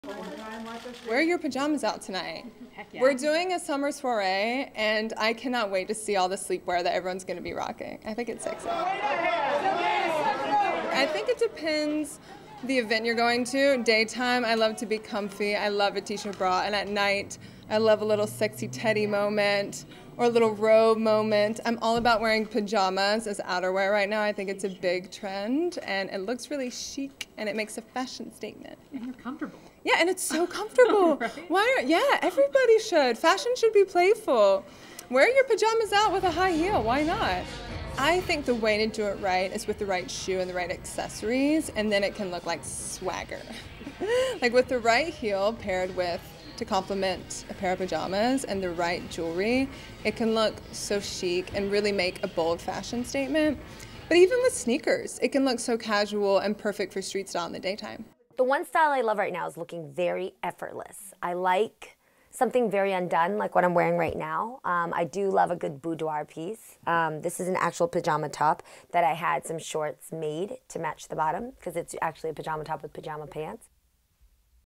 Martha Hunt says pajamas are no longer just for the bedroom
(16 Jun 2018) FOR CLEAN VERSION SEE STORY NUMBER: 4158283